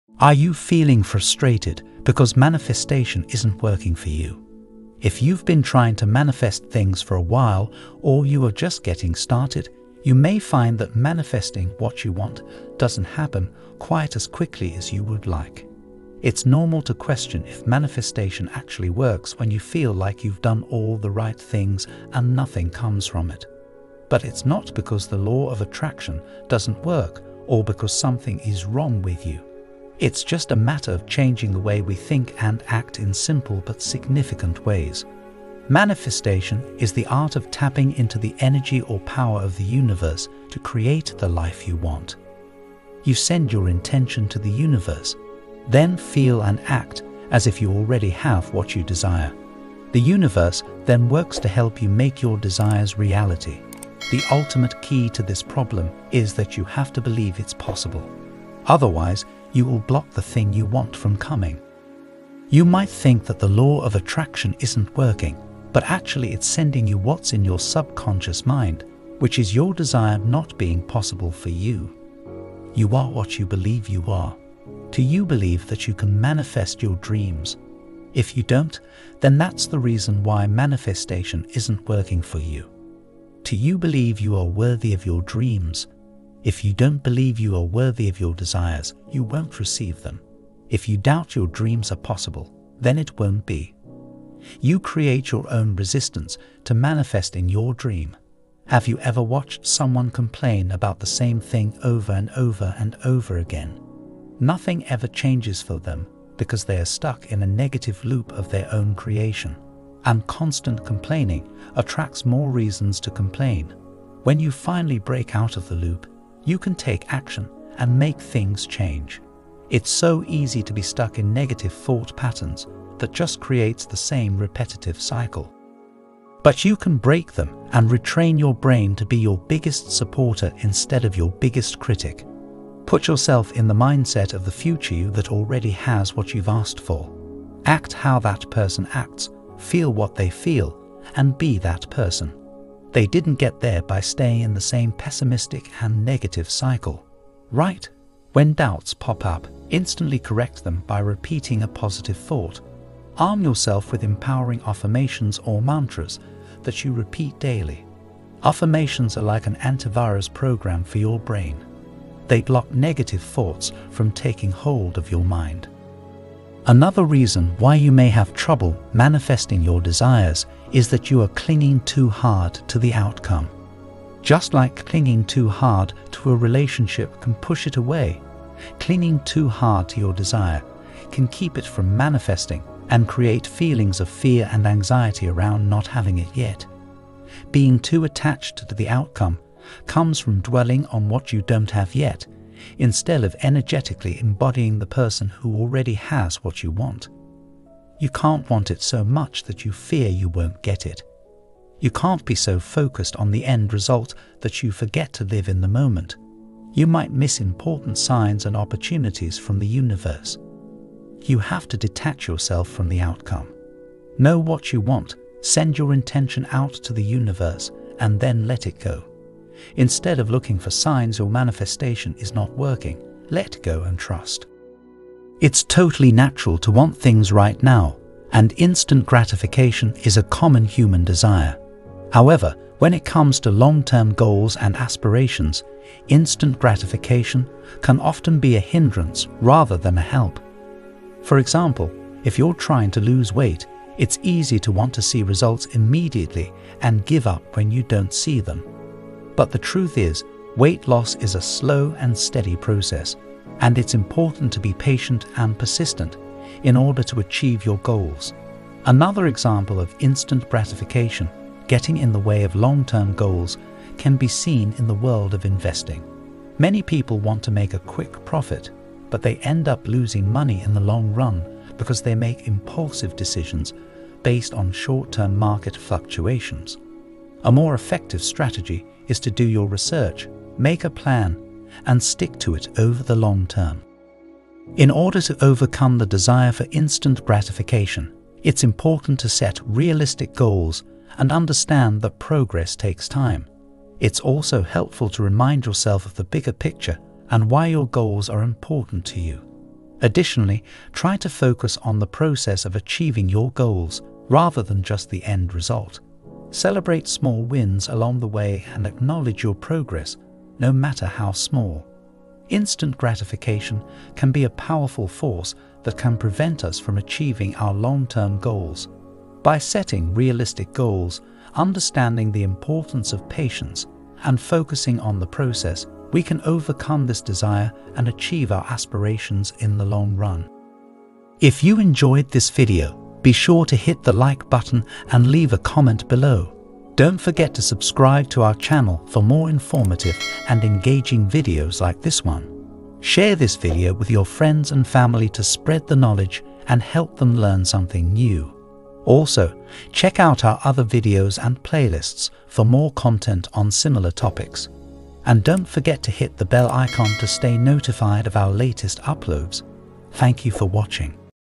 If It Hasn't Manifested Yet, THEN WATCH THIS! (Universe Wants You To Watch This!) - Neville Goddard
In this video I talk about Neville Goddard's techniques to mastering the
Law of Assumption. Join me as I explain in further detail what Law of Assumption entails. Once you master this one simple knowledge you will be able to manifest anything you believe.